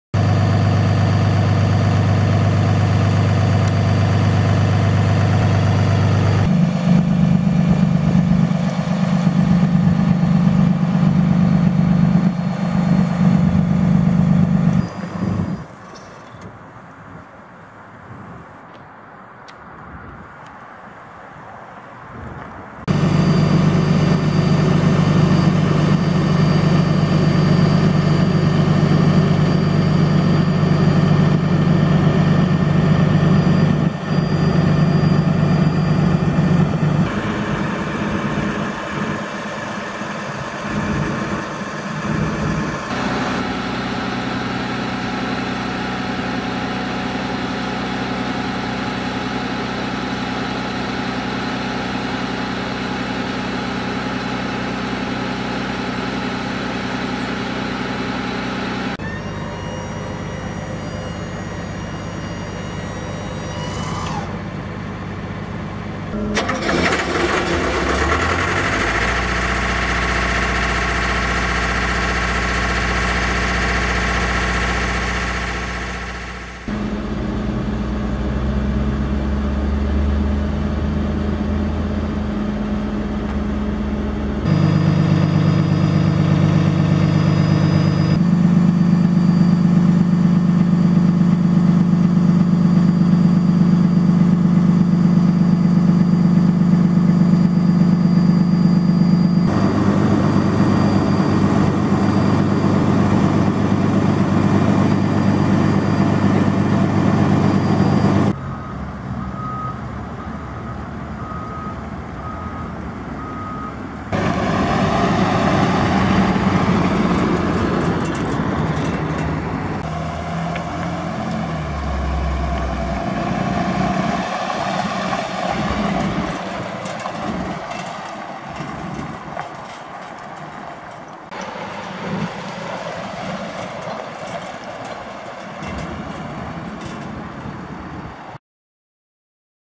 2010 Freightliner M2 106 Crew Cab (#5789)
2010 Freightliner M2 106 Crew Cab
- Service and Crane Truck
- Cummins ISB 7.3 Litre @ 240 HP
- Allison RDS 2200 Transmission
- Liftmore Crane 2700# Capacity
- Miller AirPak Welder/Compressor
- Waltco Liftgate - Only 29,266 Miles